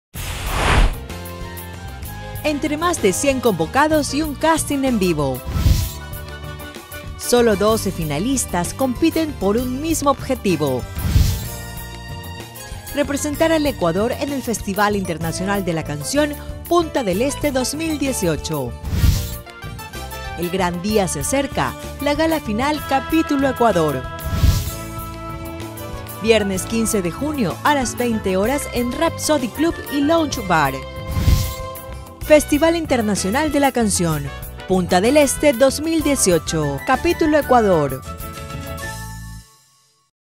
Festival Internacional de la Canción - Punta del Este 2018 - Capítulo Ecuador
Festival Internacional de la Canción
 - Punta del Este 2018 -
     Capítulo Ecuador

Continuamos con la convocatoria para seleccionar a quien nos representará en el VII Festival Internacional de la Canción - Punta del Este 2018, que se realizará en el mes de noviembre en Uruguay.

Se invita a autores, compositores e intérpretes (solistas) ecuatorianos (mayores de 18 años) para que se inscriban y envíen su demo.

El tema debe ser inédito y debidamente registrado (desde la etapa del casting. Si un tema pasa a la final sin estar registrado, será eliminado).

El cierre de inscripción es el 1 de mayo. Los calificados participarán en un casting en vivo.

 Sólo 12 de ellos pasarán a la Gala final que se realizará el 15 de junio en Guayaquil.

ARTISTAS QUE HAN REPRESENTADO AL ECUADOR EN EL FIC PUNTA DEL ESTE

2012 OLGA MARÍA OLEAGOITIA
2013 DANILO ZAM (CANCIÓN GANADORA)
2014 XAVI MORA
2015 MATEO ALAN
2016 NICOLE RUBIRA
2017 GABRIELA FRANCO (ARTISTA MÁS POPULAR)

MÁS INFORMACIÓN DEL FESTIVAL

El Festival Internacional de la Canción Punta del Este 2018 Capítulo  Ecuador, elige a  nuestro representante al Festival Internacional de la Canción Punta del Este que se realiza, desde hace seis años, en Uruguay, en el mes de noviembre. En él participan más de 20 países con temas inéditos.

En este Festival se elige:

  - Canción Ganadora

- Mejor Videoclip

CAPÍTULO ECUADOR

REQUISITOS

- Ser solista (hombre o mujer)

- Ecuatoriano/a

- Mayor de 18 años

- Inscribir sólo una canción

- La canción debe ser inédita. Quiere decir que no ha tenido difusión en medios o redes, ni ha sido grabada o publicada en un material discográfico. Debe ser totalmente nueva.

- La canción debe estar registrada. Si pasa a la final sin ser registrada, será eliminada.

- La letra no debe tener mensajes ofensivos ni temática religiosa o politica.

- El género musical es libre.

ENVÍO

Cumpliendo los requisitos y proporcionando los datos solicitados.
Envíanos vía mail:

- Inscripción (gratuita) ficha que se provee vía mail al participante, para llenar con sus datos personales
- 4 fotos (2 de cara y 2 de cuerpo completo)
- Pista de la canción con voz y sin voz en formato MP3
- Letra de la canción en archivo Word

FECHA DE ENTREGA Y SELECCIÓN

-Enviar todo hasta el 1 de mayo.
Todas las canciones entrarán a un proceso de evaluación. Los seleccionados serán convocados a un casting en vivo en mayo. Sólo 12 participarán en la Gala final que se realizará el 15 de junio.a las 20h00 en Rhapsody Club & Lounge Bar (Urdesa Central).

El ganador nos representará en el Festival Internacional de la Canción - Punta del Este 2018 que se realizará en el mes de noviembre.en Punta del Este Uruguay..

PREMIOS

El ganador o ganadora del Capítulo Ecuador obtendrá por parte de la organización:

- 1 pasaje ida y vuelta a Uruguay para su participación.

-En Uruguay, todos los gastos pagados (hospedaje, alimentación y transporte)

- VÍdeo musical del tema inédito (grabación produccion y edición). El vÍdeo es uno de los requisitos para participar en Uruguay.

- $300 para viáticos

-PremioFestival Internacional de la Canción
     Capítulo Ecuador s de los auspiciantes